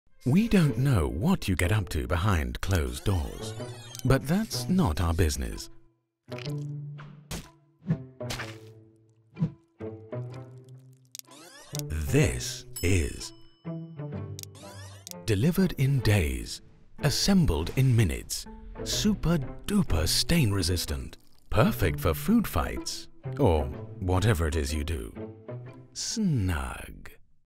Snug - The Sofa in a Box Company | Turmeric Velvet
What Snuggers ask for, Snuggers get...and it’s back! 🎉🍝

Since the dawn of time, Snuggers have been asking for this majestic colour... so here it is, straight from our imaginations into your home.

A perfect mustard-like shade for those who like a lil’ spice. We think this baby's the best colour yet. Flop down and get super comfy!

Turmeric Velvet has fast & free delivery, can be assembled in minutes and is crafted from our easy-care, stain-resistant, vegan materials. Making this colour PERFECT for food fights. It’s like... mustard on mustard, who gives a hoot?

G'on - get messy. 😈